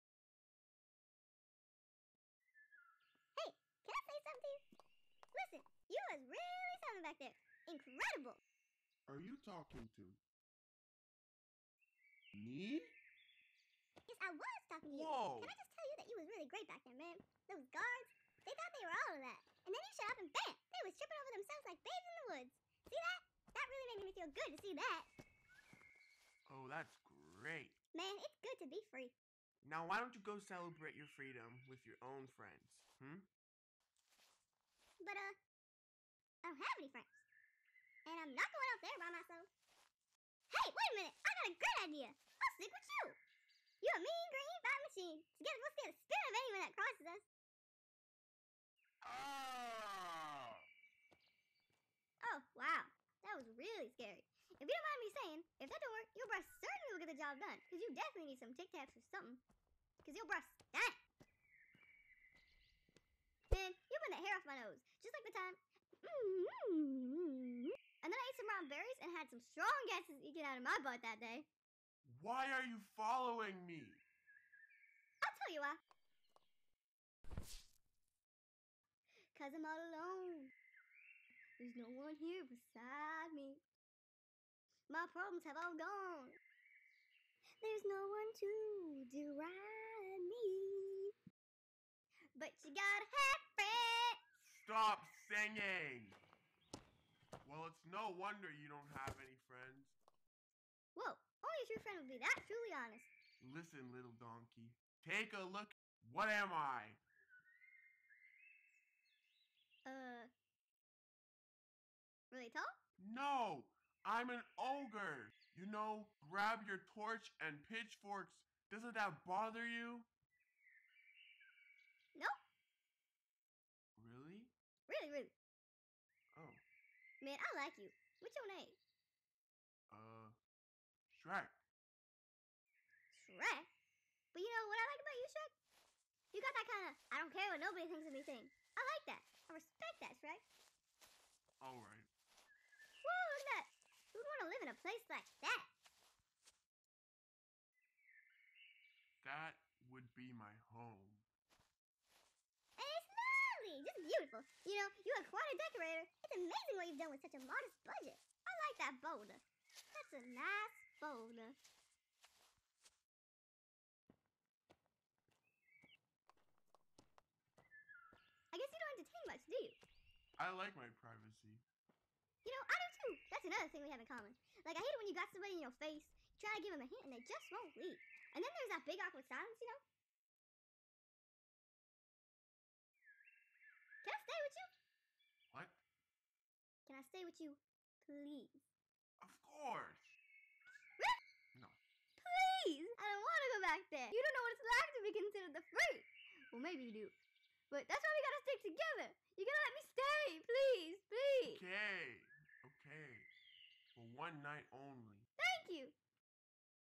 Grade 12 Comm Tech Sound Editing Assignment - Shrek
St. Mary Communications Technology Sound Editing Assignment...Students replaced the original audio from the animation clip...re-recorded and editing their own audio and sound effects into the video clip...really fun...By Bryanne